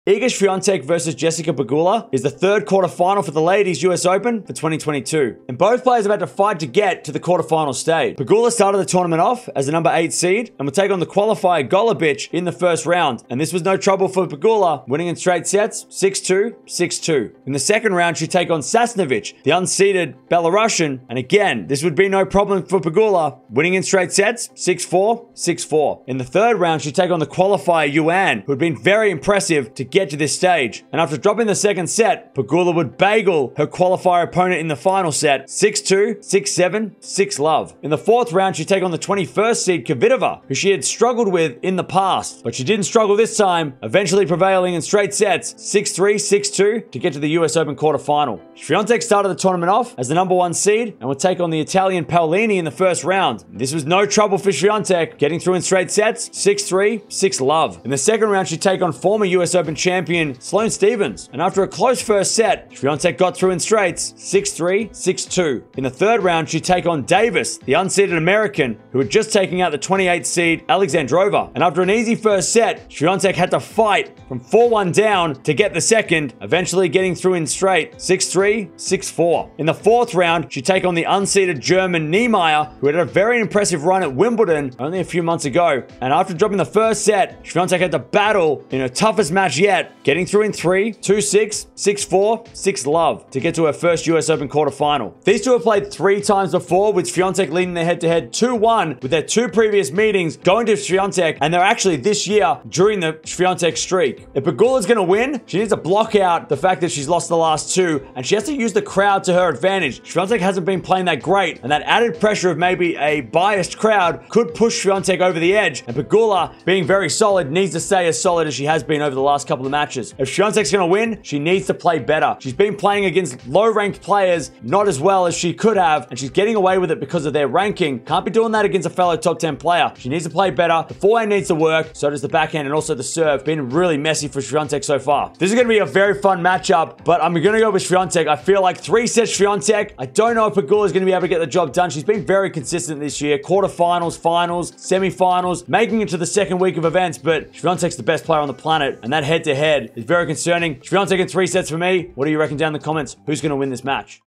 Iga Swiatek vs Jessica Pegula | US Open 2022 Quarter Final Preview | Tennis Talk News
Iga Swiatek will take on Jessica Pegula in the fourth quarter final of the US Open for 2022. These two have faced off twice this year with Iga getting the wins. Who will make it to their first US Open Semi Final?